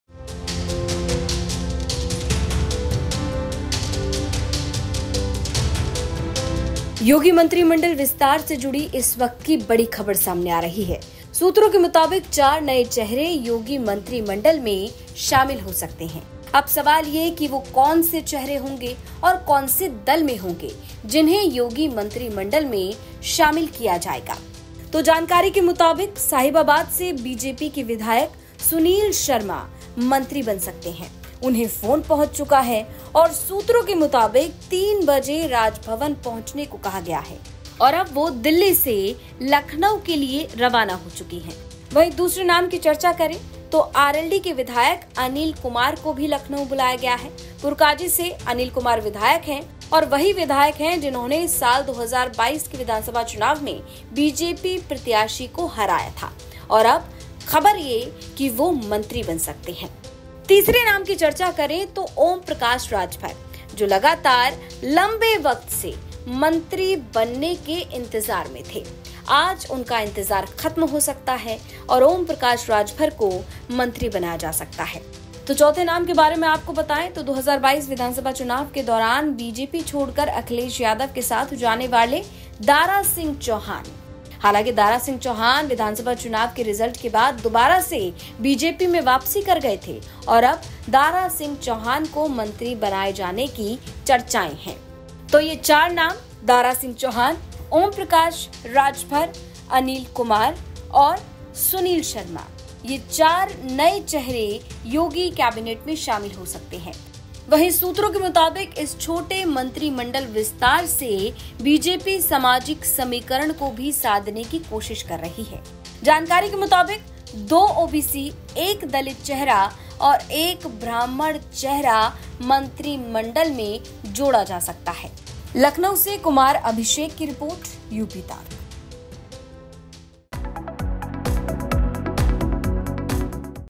Yogi Cabinet Expansion : ये 4 नए चेहरे बनेंगे योगी सरकार में मंत्री, नाम जानकर हैरान हो जाएंगे!
आज यानी 5 मार्च को योगी कैबिनेट का विस्तार हो सकता है. सूत्रों के मुताबिक, इस बार 4 नए चेहरे योगी मंत्रिमंडल में शामिल हो सकते हैं. रिपोर्ट में जानिए आखिर कौन योगी कैबिनेट में शामिल हो रहा है..